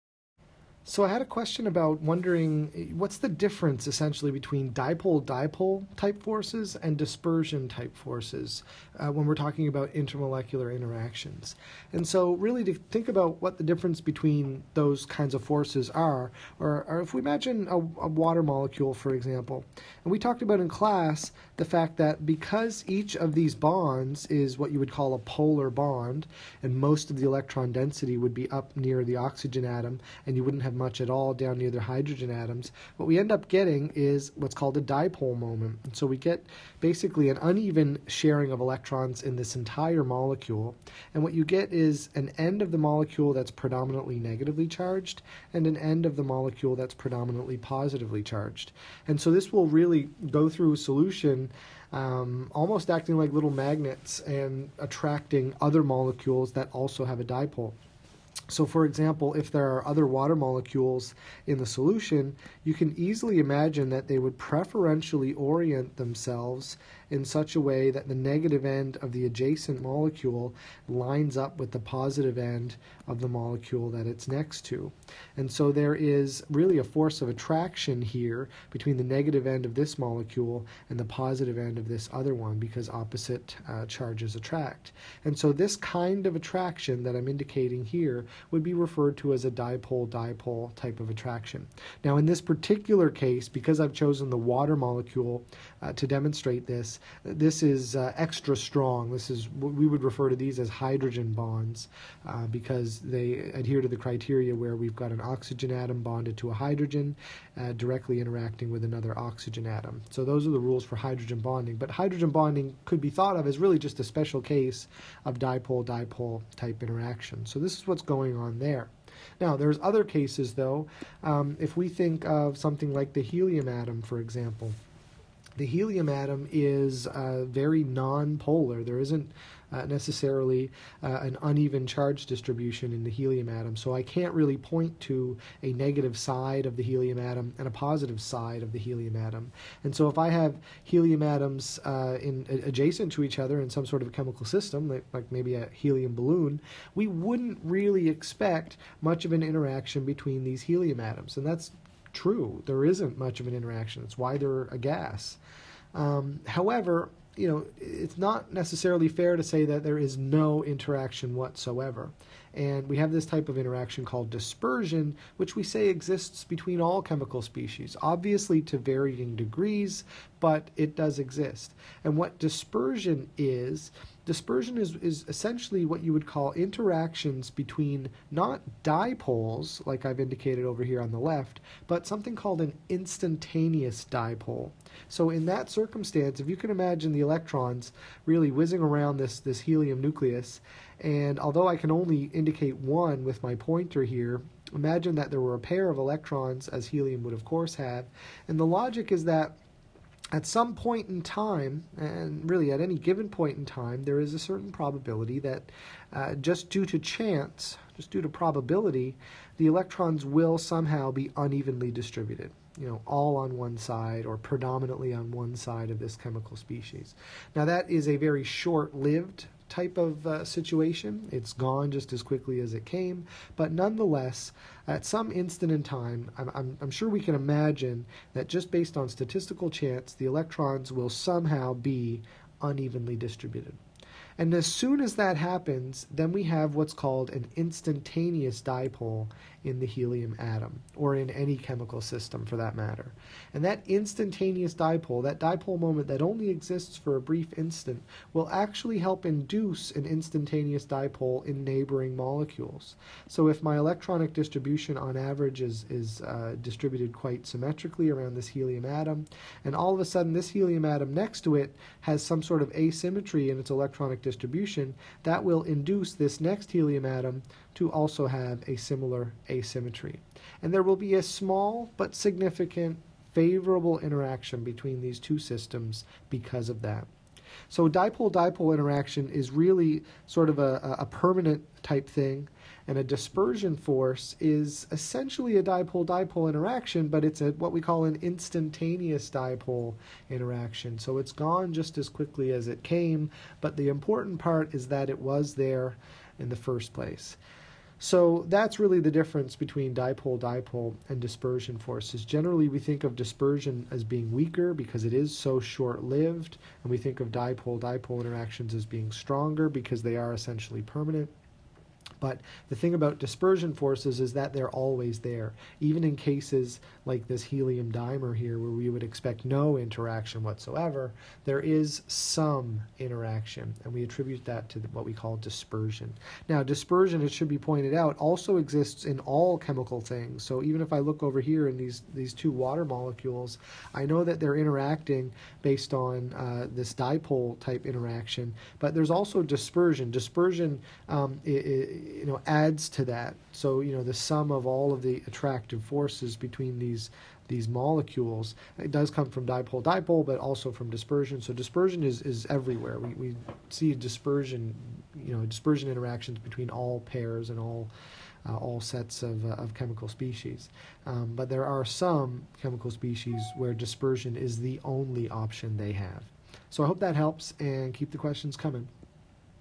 Dipole-dipole and dispersion forces
What is the difference between dipole-dipole forces and dispersion forces?